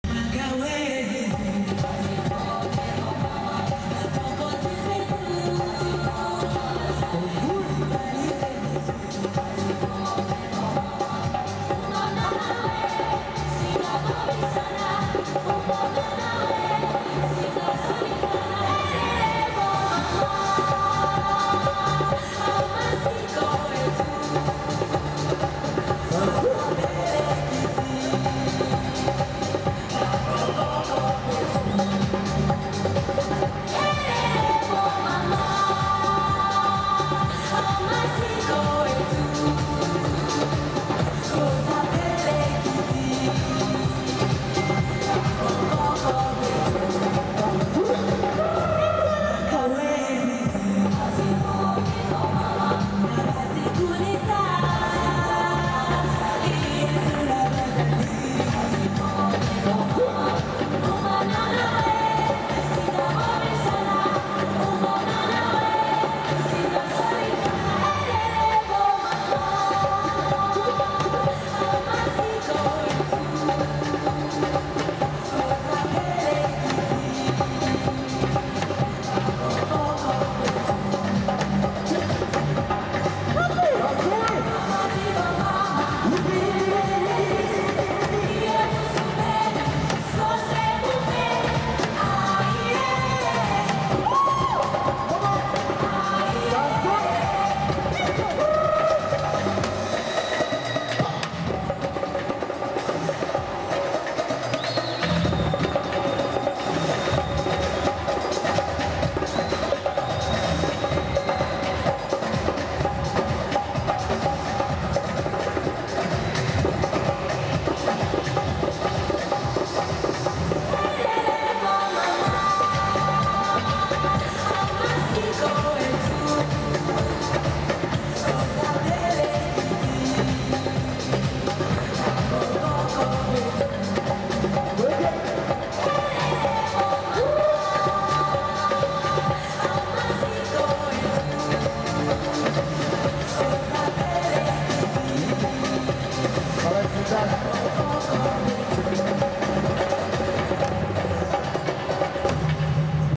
Искромётный номер! Ребята из Африки зажигают на концерте в Астане. Дни Всемирной выставки ЭКСПО
Искромётный номер!  Ребята из Африки зажигают на Всемирной выставке ЭКСПО 2017 в Астане. Солистка супер!  Зарядившись живой энергетикой, зрители пошли в пляс!)